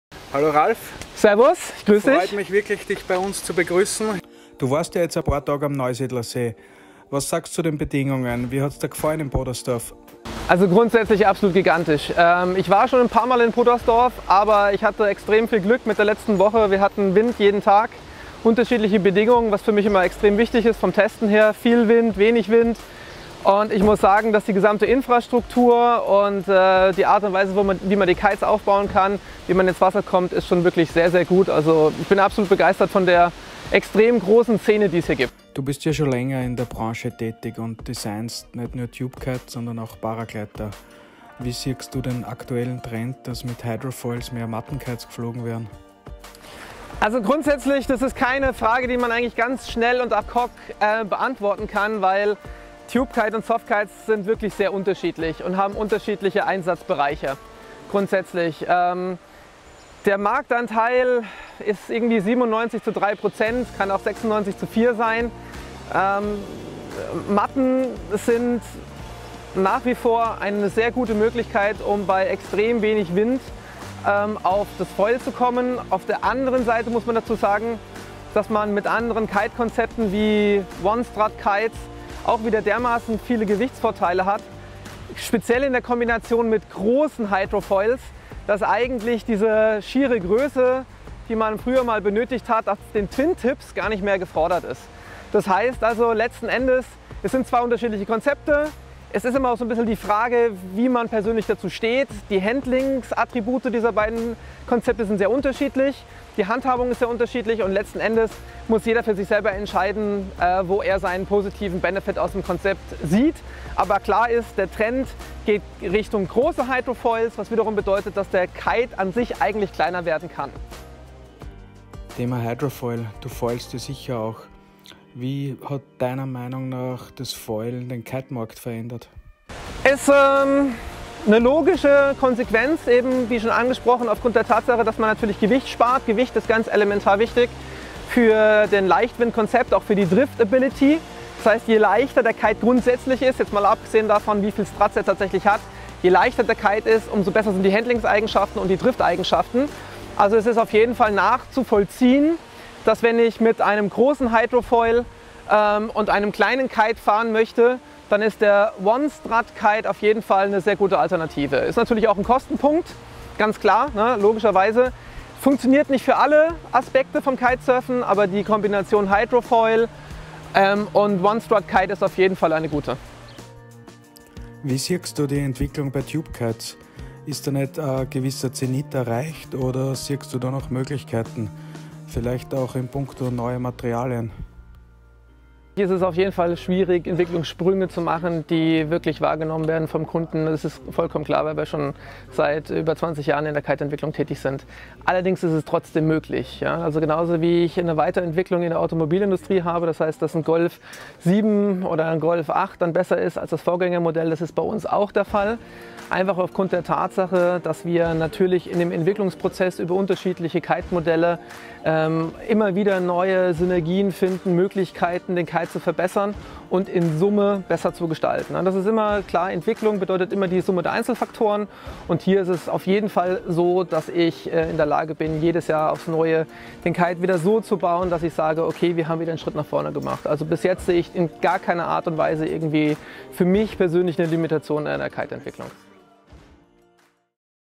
Kitedesigner Ralf Grösel im Techtalk (mit Jo von usd.at)
Ralf Grösel ist Kitedesigner bei Duotone Kiteboarding. Er ist verantwortlich  für die Kite Linien Vegas, Dice, Evo und Juice. Seit seinem 14ten Lebensjahr war er fasziniert von Paragleitern und „flexiblen Flügeln“. Er hat autodidaktisch CAD Design und Flügeldesign gelernt und schon für einige bekannte und erfolgreiche Firmen in der Branche gearbeitet. Als Gründungsmitglied von Flysurfer Kites und Skywalk Paragliders fing er 2001 an in der Kitebranche. Kitern, die schon länger dabei sind, ist auch der X3 von Naish sicher noch ein Begriff für den Ralf damals bei Naish verantwortlich war.
Seit 2005 ist er bei Boards & More und war anfangs für die Snowkites verantwortlich. 2007 entwickelte Ralf den ersten EVO, der am Markt extrem gut ankam! Etwas später übernahm er die Entwicklung des VEGAS von Ken Winner, den er in weiterer Folge mit Aaron Hadlow maßgeblich weiter verbessert hat. Auch der extrem erfolgreiche Kite DICE stammt von Ralf, sowie der NEO und seit 2017 auch der JUICE.
Seine enorme Erfahrung mit Kites & Flügeln/Paragleitern spürt man in jedem Duotone Kite, den er designt hat. Ralfs Designs zeichnen sich auch durch seinen starken Drang nach Perfektionismus, sowie seine extrem professionelle Arbeitsweise aus. Das konnten wir auch live beobachten als er bei uns am Strand Prototypen getestet hat. Sein Zugang ist „nicht eine Lösung für ein Problem zu finden – sondern die BESTE Lösung für ein Problem zu finden“.

Additional Footage: B&M; Chris Zarfl